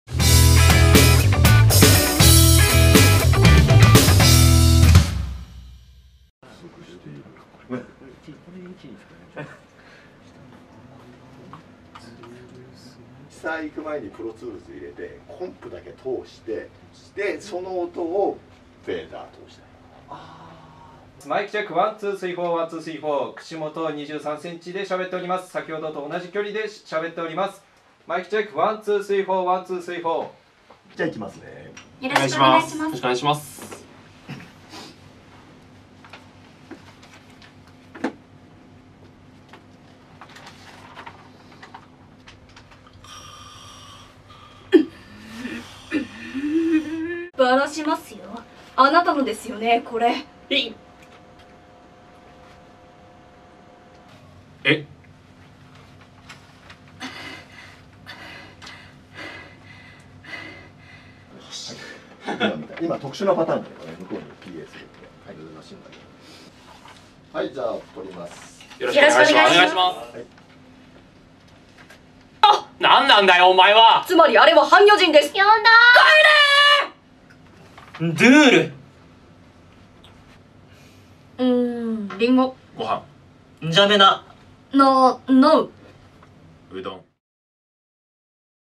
放送音響科「アニメ音響実習」の様子
東放学園専門学校・放送音響科の授業「アニメ音響実習」の様子。